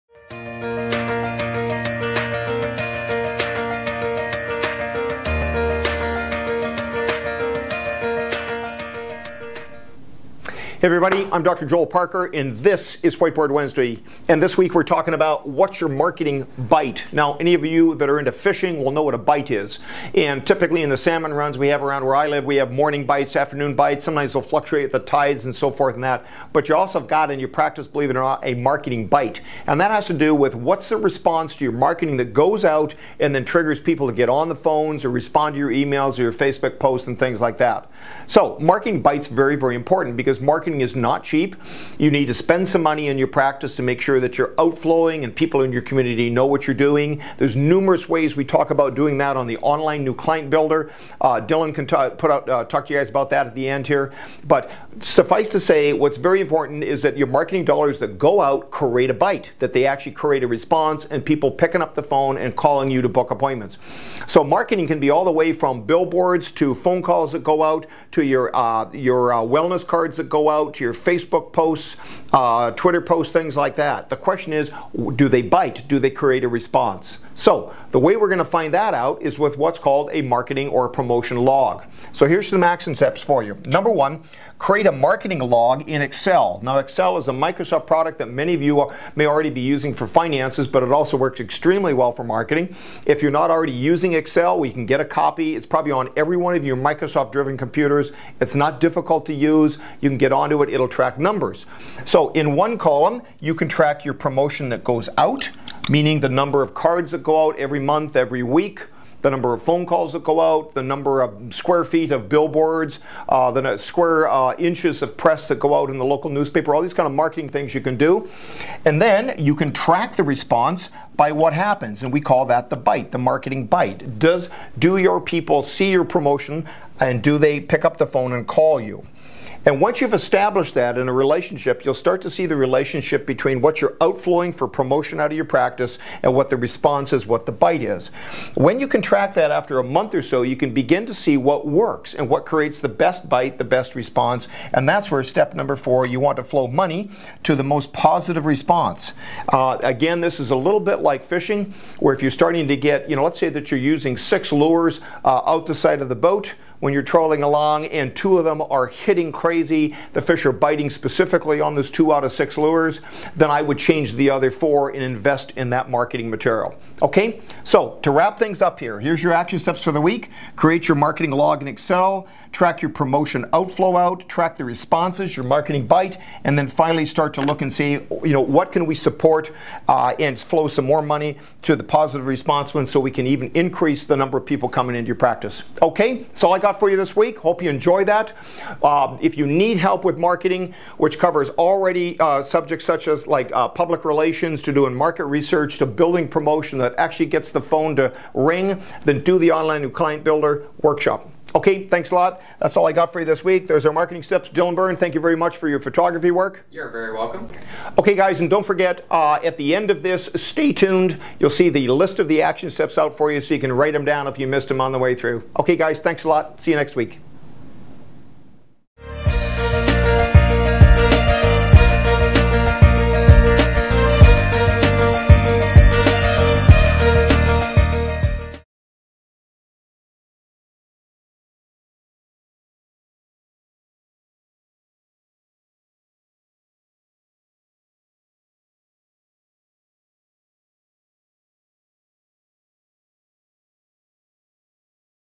What's Your Marketing "Bite?"
Much like in fishing, there are certain factors that will influence your chances of getting a "bite" when it comes to clients responding to your promotion. In this episode of Whiteboard Wednesday, Dr. Joel Parker discusses how you can maximize your marketing budget by figuring out where you are getting the most response or "bite" from and putting your money there.

Do the Online New Client Builder and learn more about how to pack your practice with new clients!